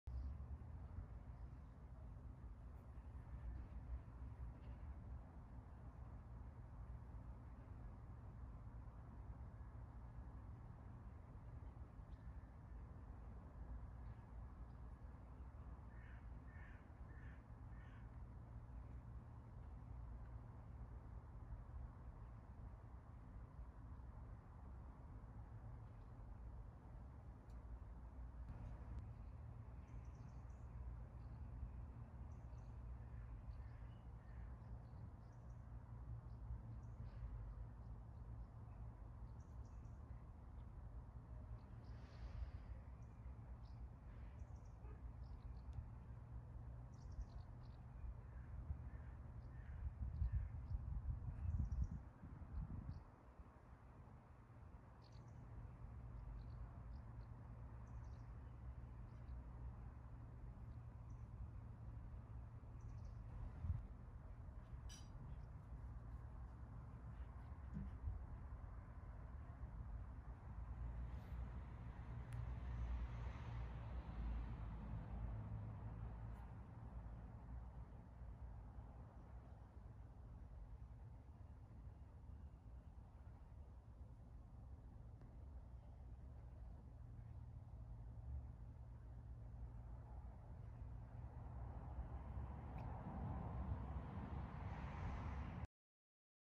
Push up 101. How to start. Bodyweight strength basics.
Go higher and work your way down.
Set up strong and make the entire movement count.
I’m a bang for your buck kinda girl. Nerd out on the details = more gainzzz.
Slow down, feel what’s going on and make it pretty. Every quality repetition adds up.
You’ll gain much more body awareness and strength in 1 good form push up than 10 sloppy ones. Trust me science 😆
More on Instagram @inverted_g